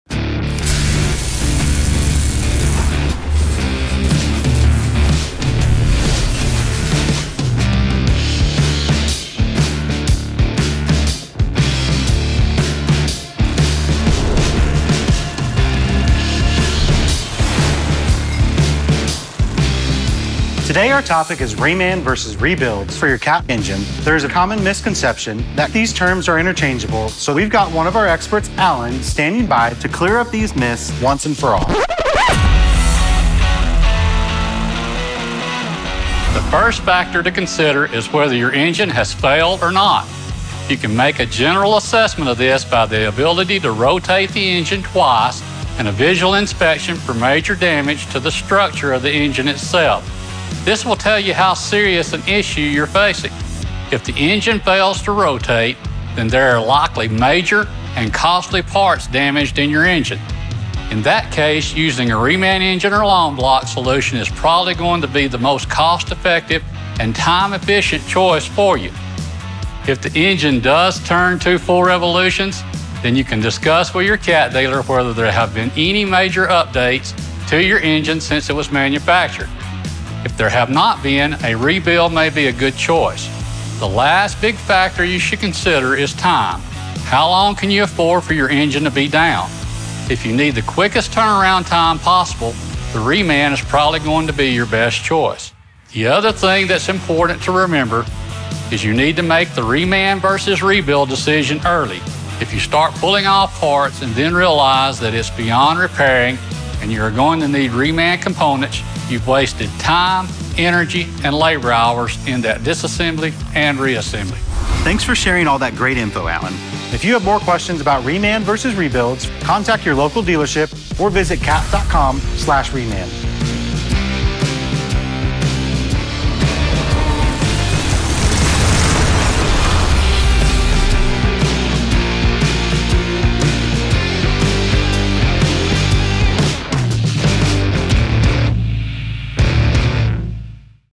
Remanufacturing vs. Rebuild - How Do I Choose For My Engine? | Myth Buster Minutes | Cat® Reman
Think that buying new or getting a rebuild are your only options for your engine? The good news is that you have choices. There are a number of repair options for your engine – two of which are rebuild and Cat Reman. The question then becomes… which option represents the best value for your needs? Our experts explain the differences here, but you can always discuss repair options with your local authorized Cat dealer.